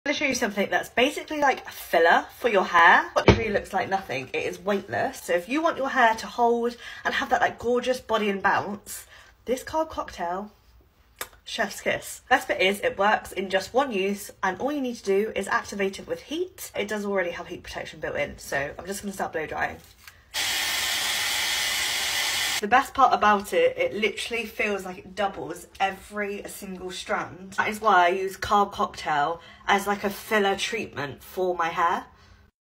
Get Thicker, Fuller Hair Instantly | The Ultimate Hair Volumizing Solution
Color Wow Carb- infused Dream Cocktail is literally like filler in a bottle for your hair! Packed with carbs, this formula forms a 3-D mesh that wraps around your strands to fatten them up, Your hair will be WAY thicker after 1 blow dry!